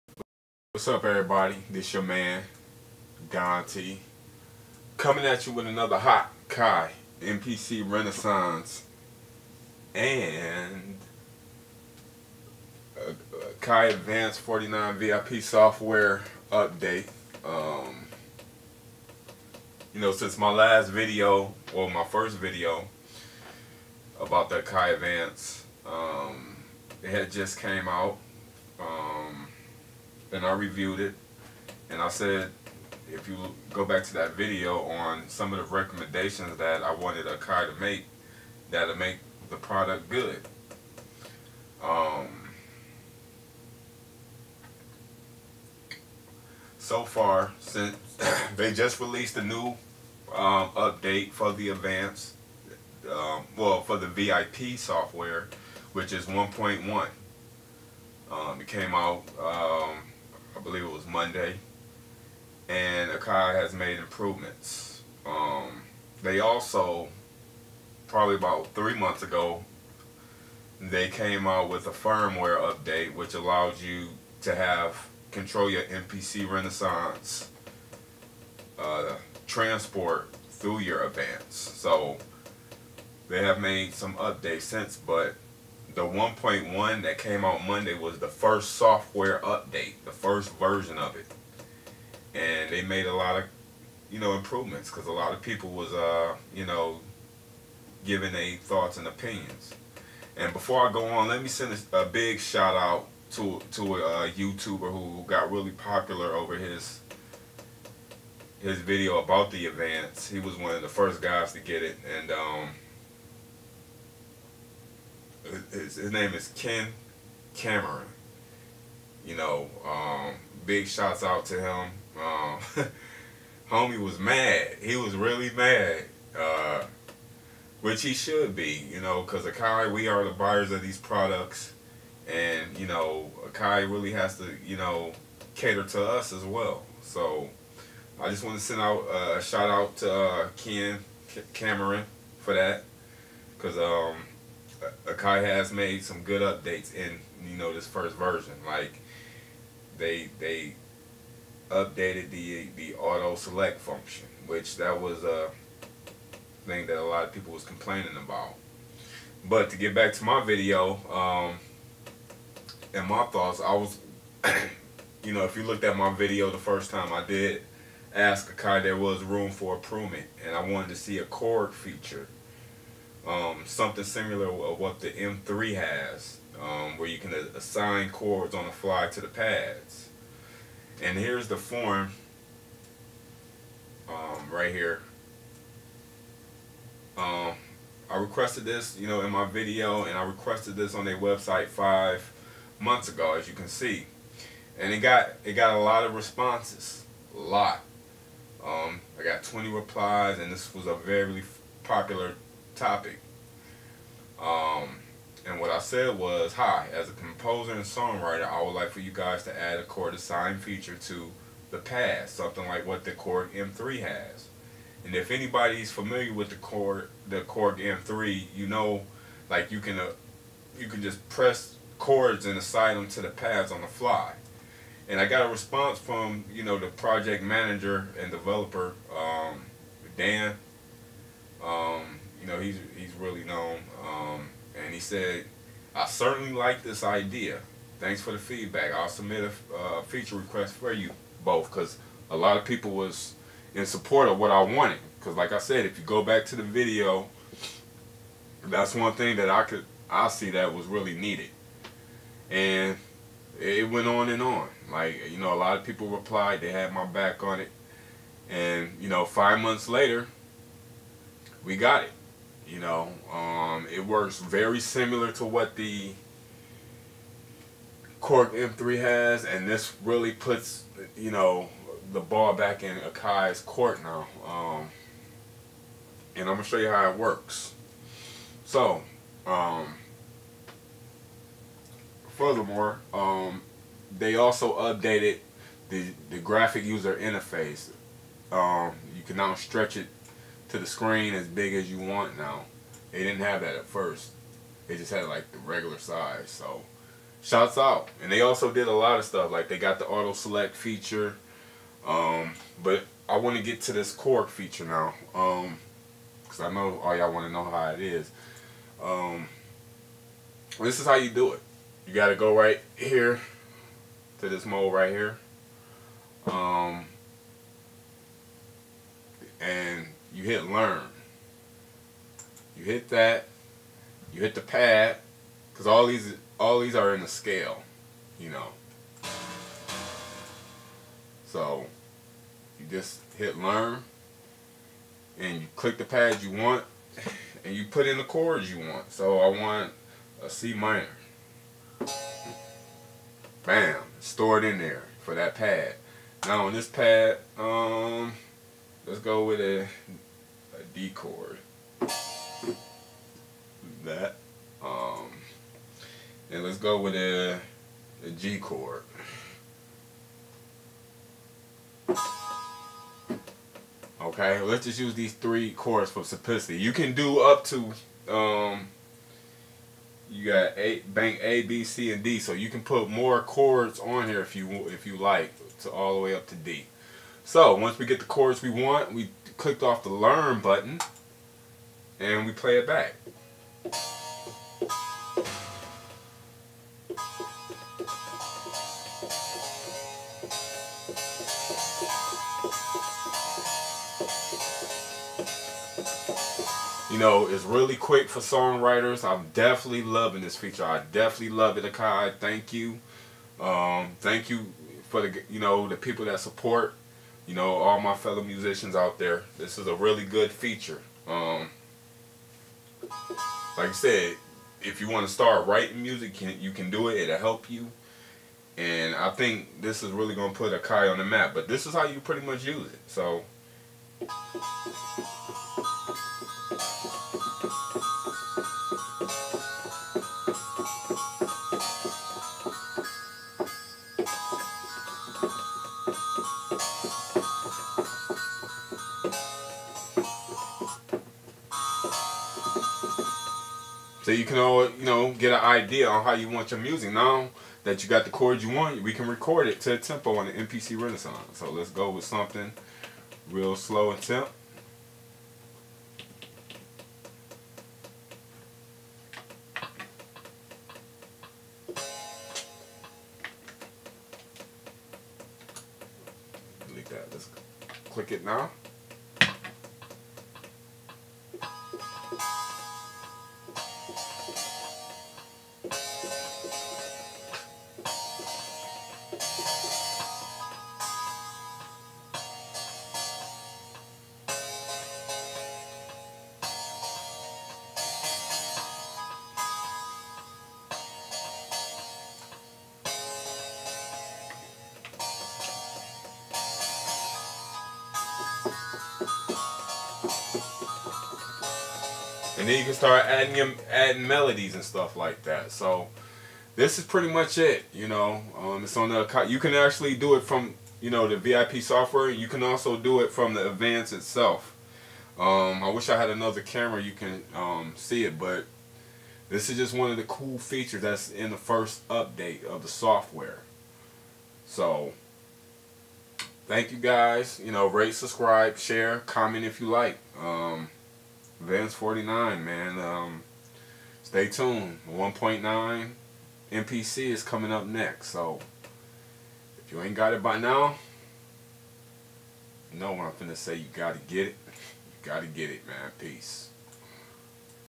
Akai Advance 49 Vip software chord feature
This video shows you the chord feature in vip software 1.1 update